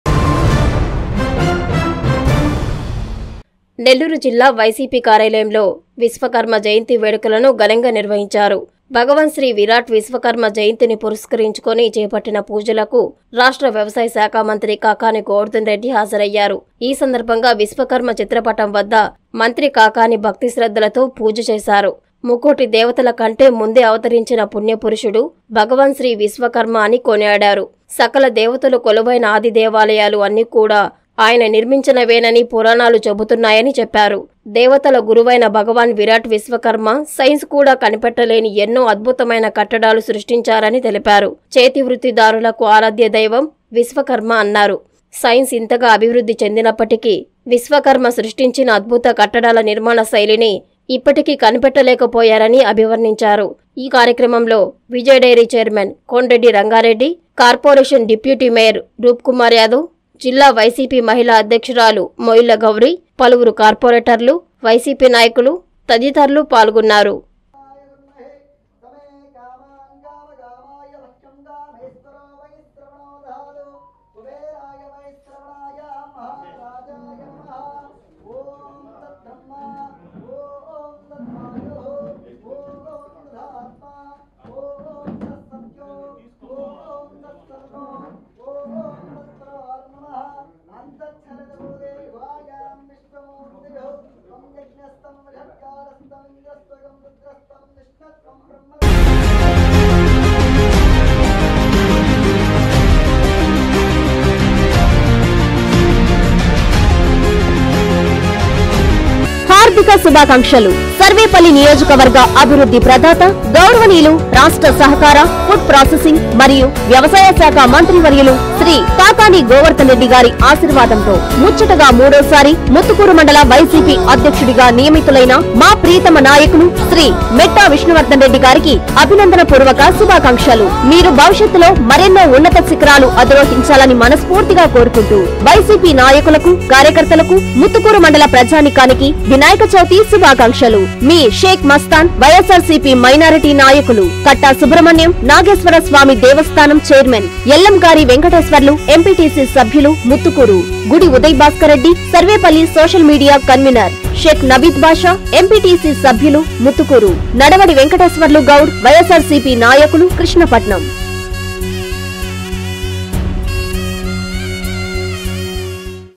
విశ్వకర్మ జయంతి వేడుకల్లో మంత్రి కాకాణి | Min Kakani at Vishwakarma Jayanti | Nellore YCP
- నెల్లూరు వైసీపీ జిల్లా ఆఫీస్‌లో విశ్వకర్మ చిత్ర పటానికి పుష్పాంజ‌లి